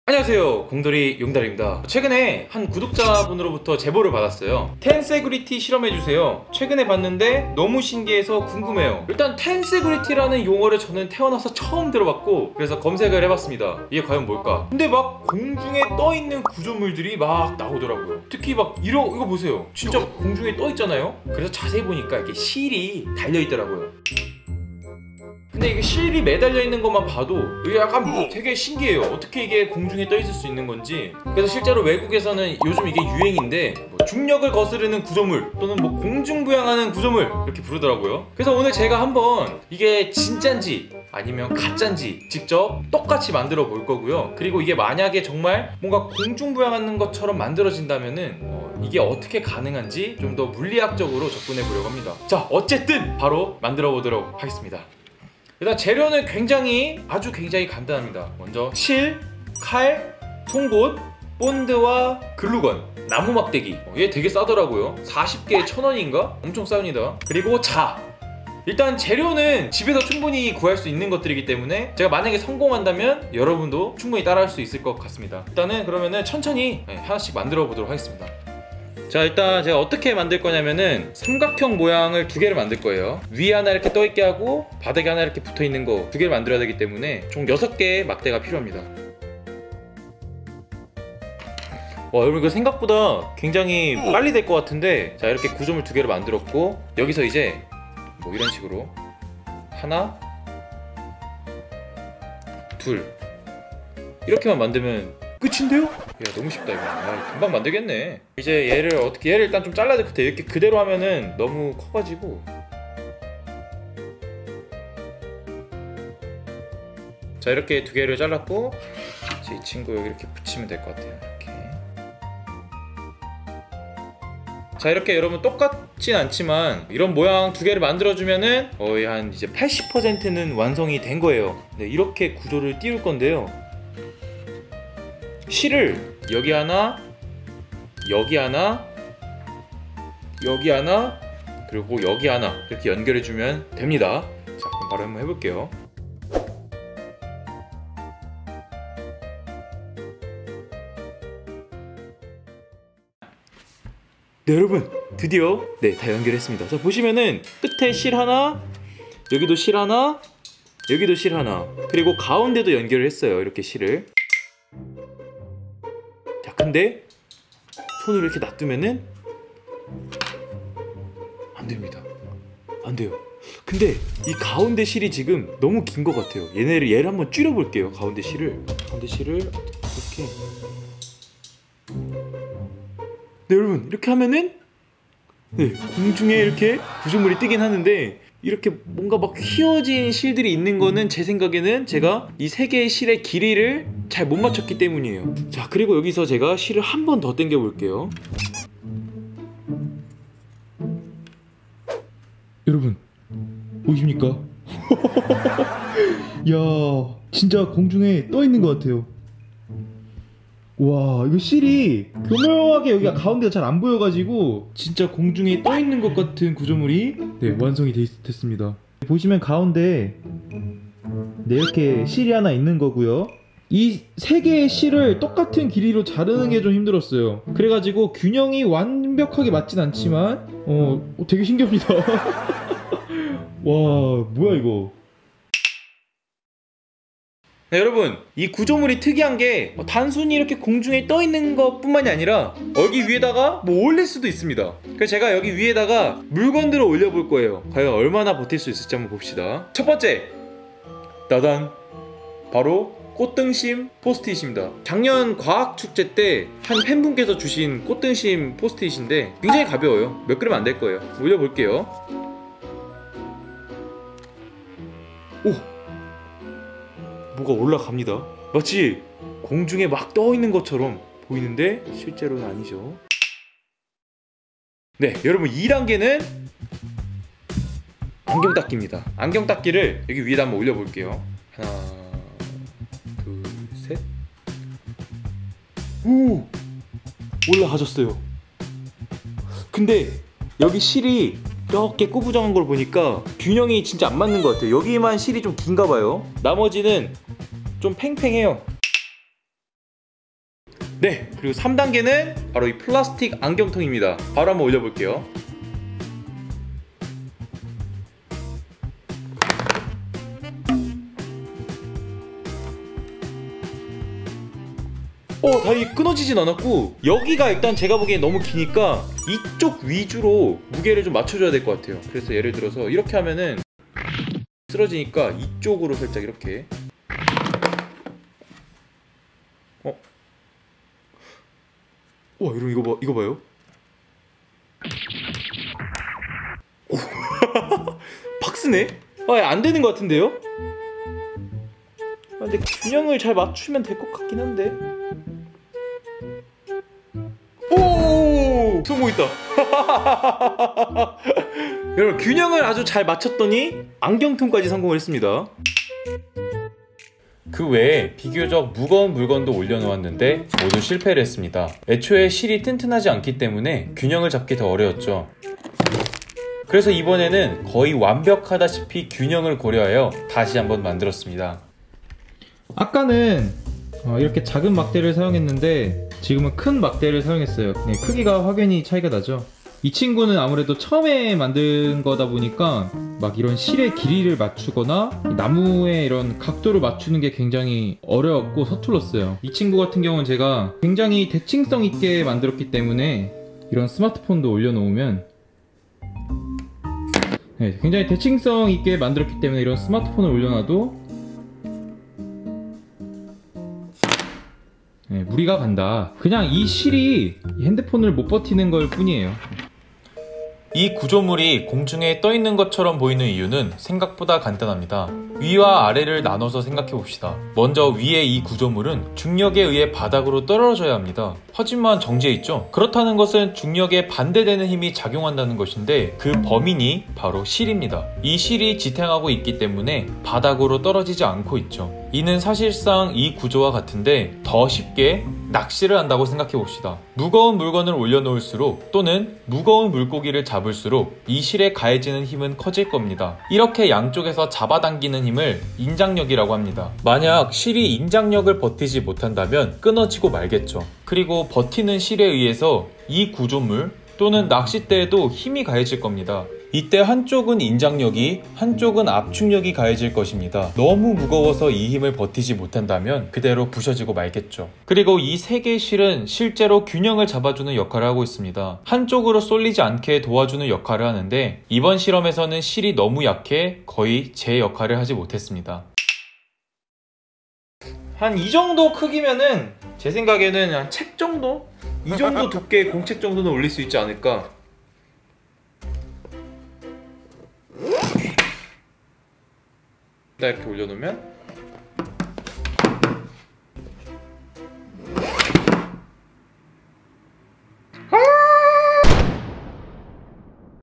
외국에서 핫한 중력을 거스르는 구조물.. 과연 사실일까?
이번 영상은 Tensegrity에 대한 영상입니다. 그런데 영상 흐름상 Tensegrity에 대한 설명을 거의 하지 못했네요.

Tensegrity = Tensional Integrity의 축약어로 1962년에 '버크민스터 플러'가 처음으로 미국 특허로 등록했습니다. 영상에 등장하는 구조물은 Tensegrity의 하나일뿐이죠. 궁금하시다면 영상 시청해주세요~